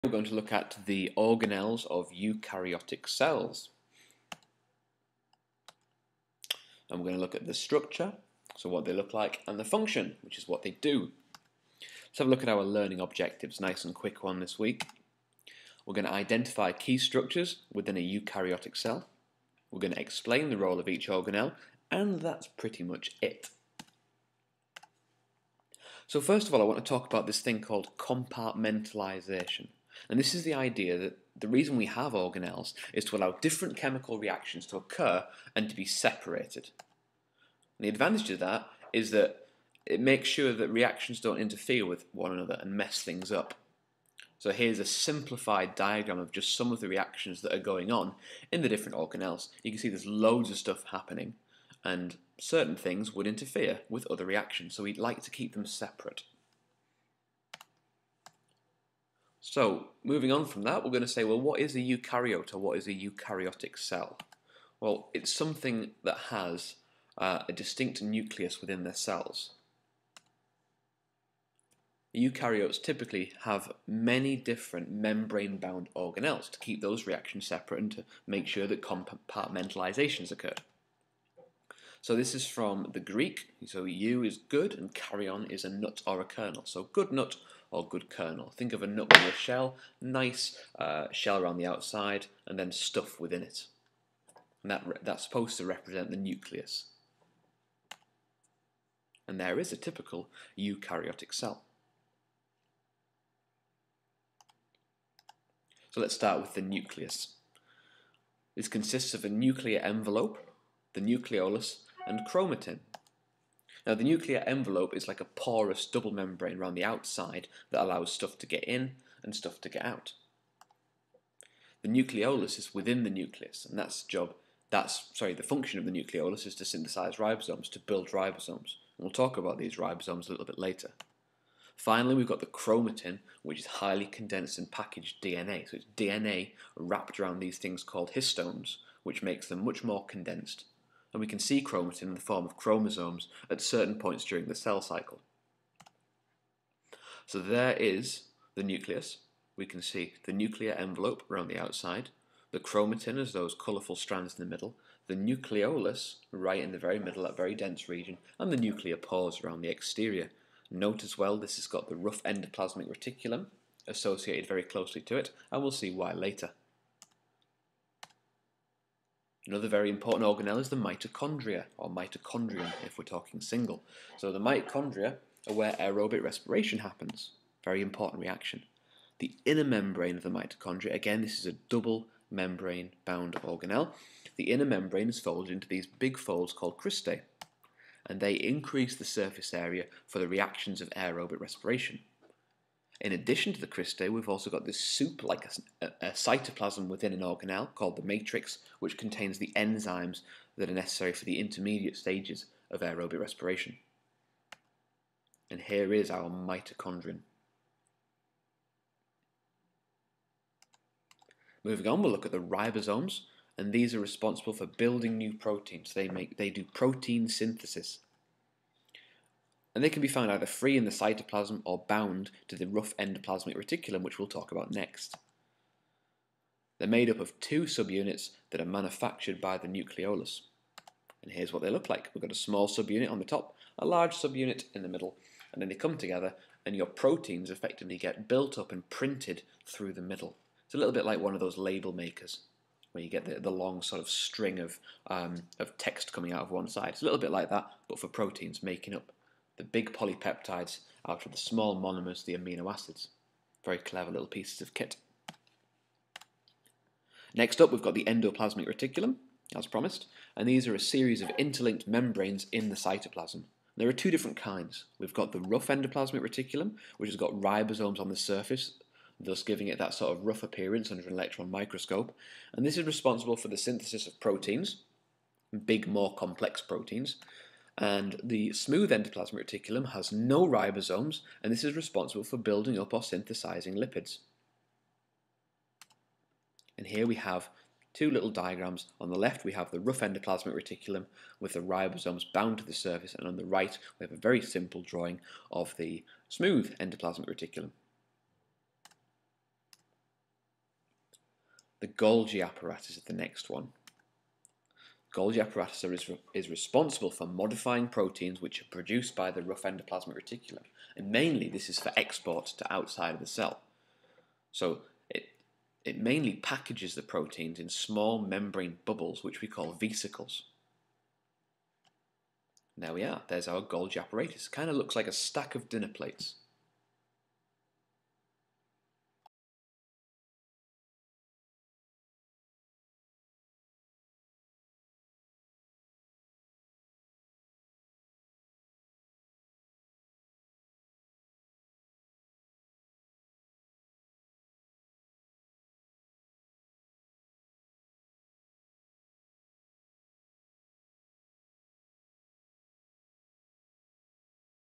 Eukaryotic Organelles
A quick tutorial, guiding you through what is meant by 'eukaryotic cell', a quick note on compartmentalisation, and the major organelles.

Made for AQA AS Biologists